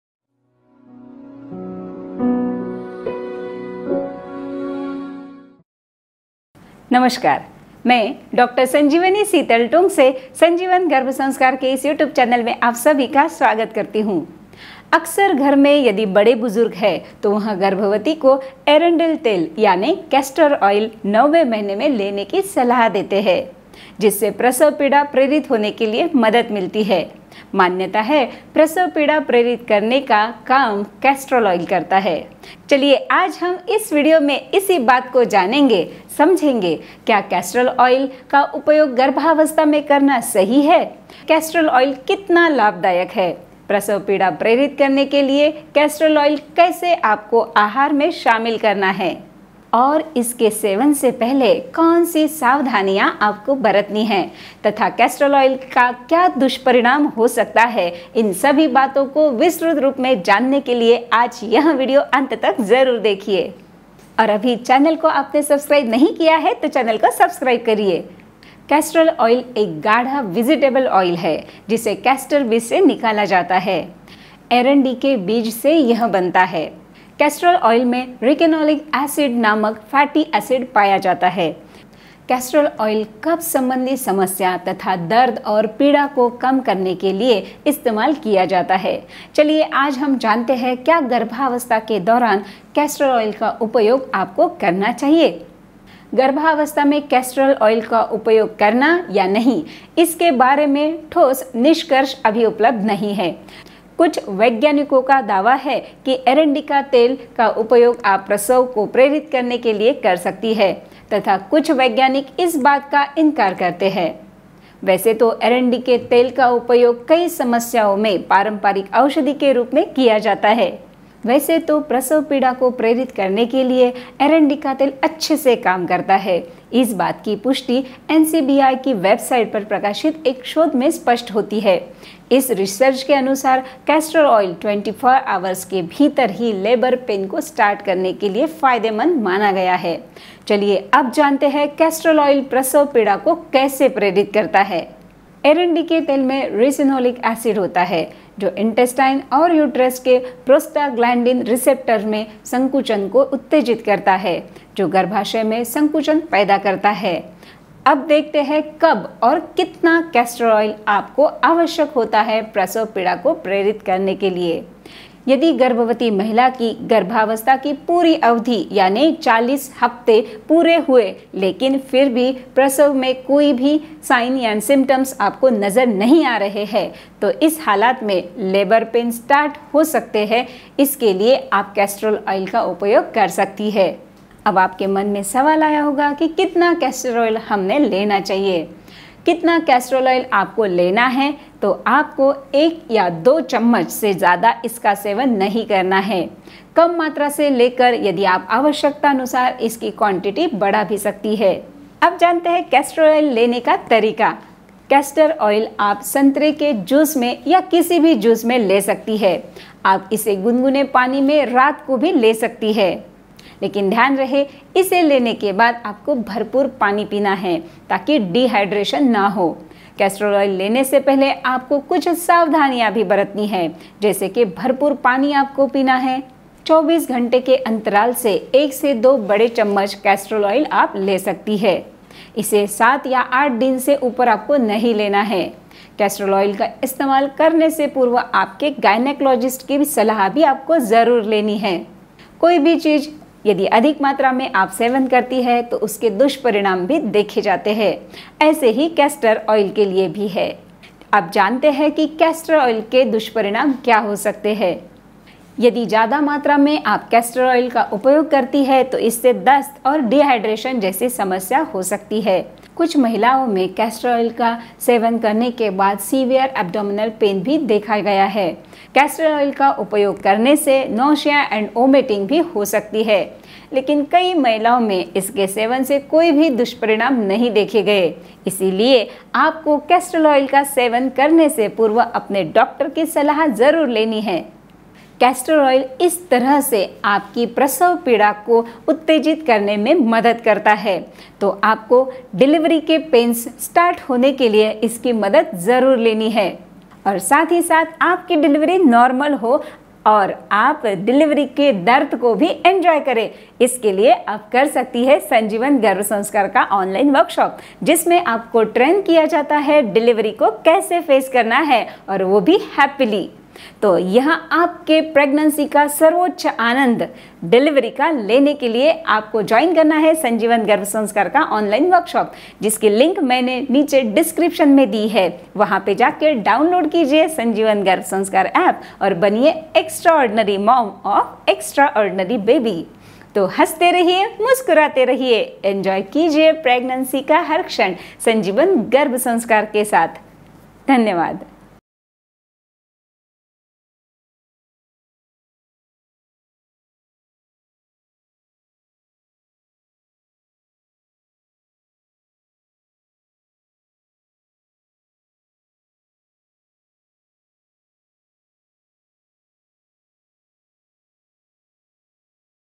अरंडी का तेल से क्या होंगी नार्मल डिलीवरी ? I Castor oil during pregnancy
इस लिंक पर क्लिक कर संजीवन गर्भ संस्कार ऐप डाउनलोड करें।You can download the Sanjivan Parenting app

नमस्कार, मैं डॉ. संजीवनी शीतल टोंगसे, संजीवन गर्भसंस्कार के इस युट्यूब चॅनेल में आप सबका स्वागत करती हूँ।
अरंडी का तेल से क्या होंगी नार्मल डिलीवरी ? I Castor oil during pregnancyइसके बारे में हम आज के व्हिडिओद्वारा जानकारी लेंगे!